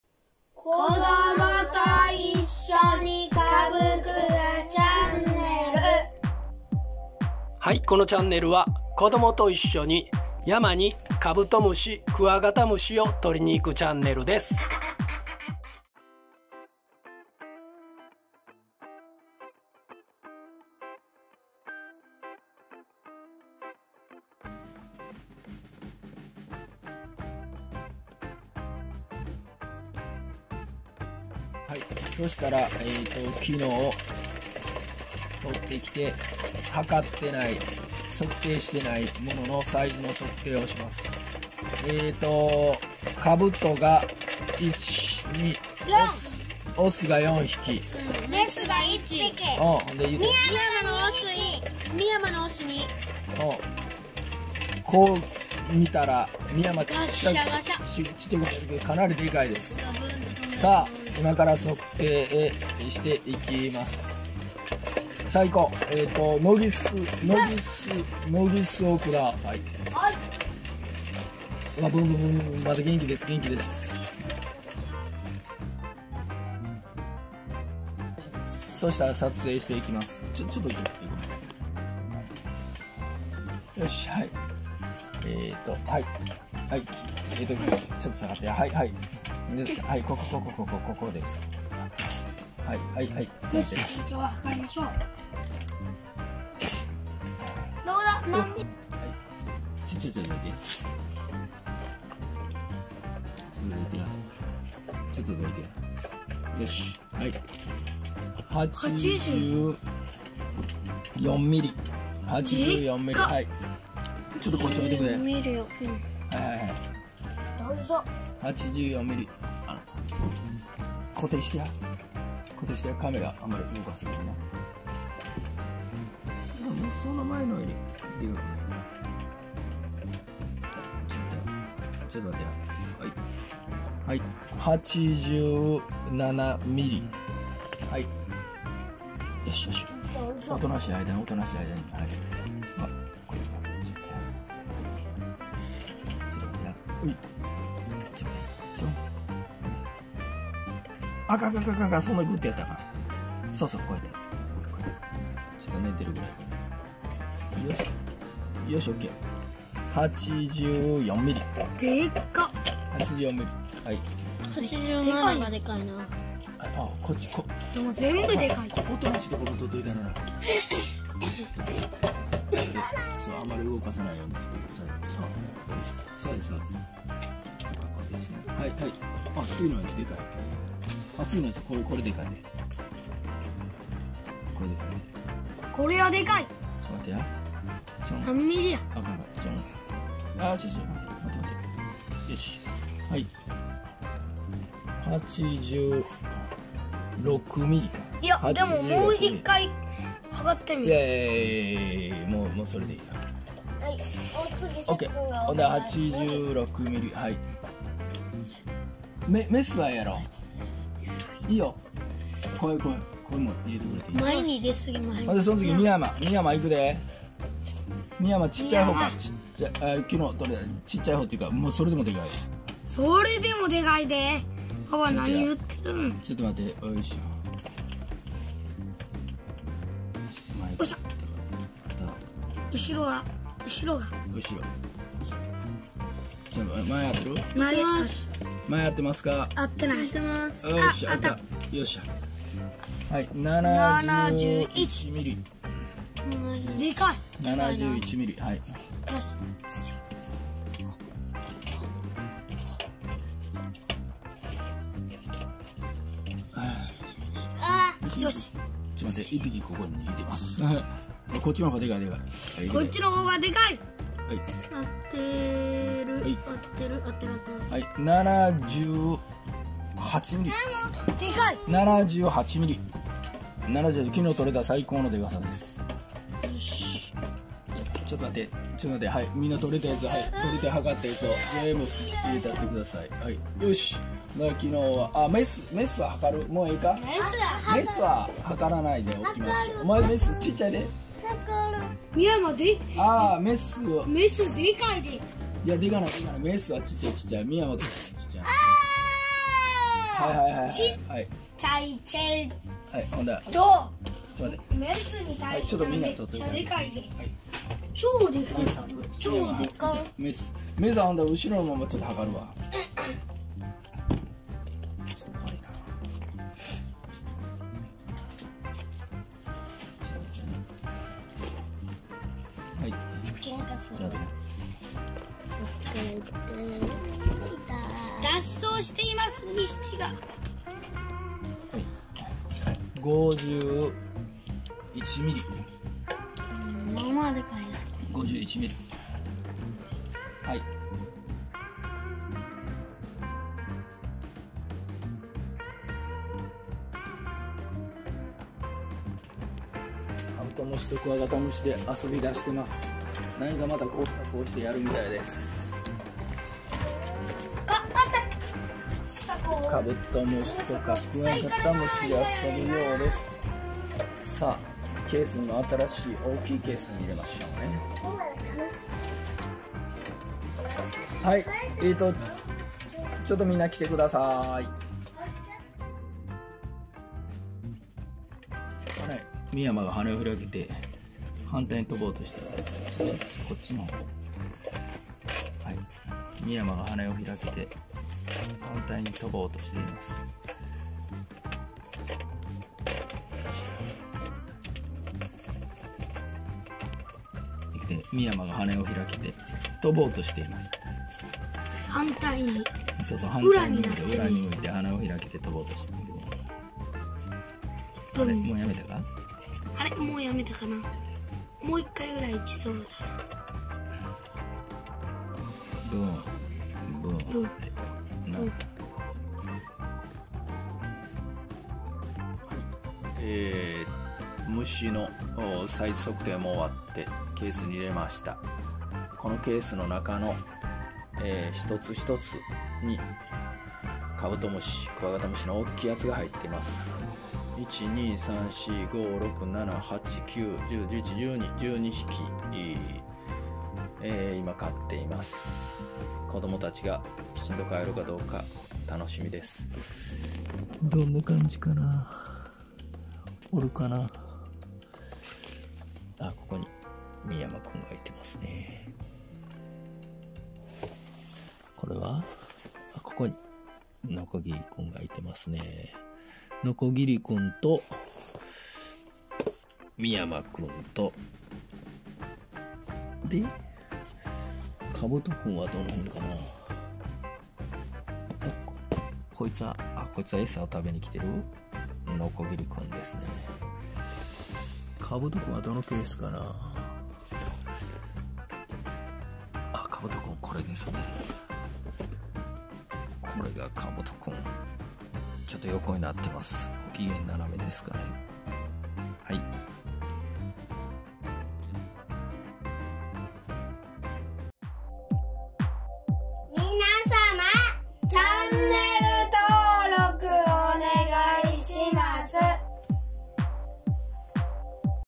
2020年 カブト87㎜、ミヤマ78㎜ サイズ測定 7月31日採集分
子どもと一緒に、大阪近辺の山に行き、
クワガタ・カブトがGETできるか検証していきます。

失敗しながらも、
大きなクワガタやカブトが取れるとうれしいです。